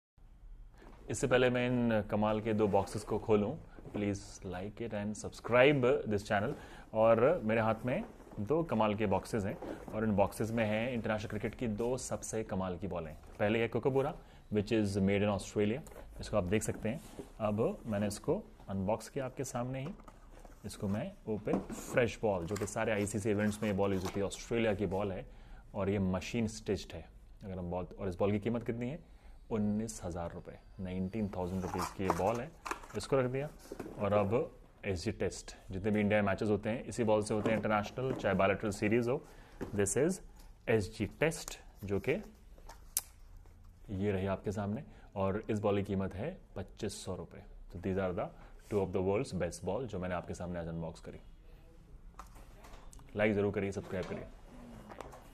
SG Test or Kookaburra - Which ball is better?
Reviewing two of the best balls in Cricket - SG Test and Kookaburra. Tell us your favorite.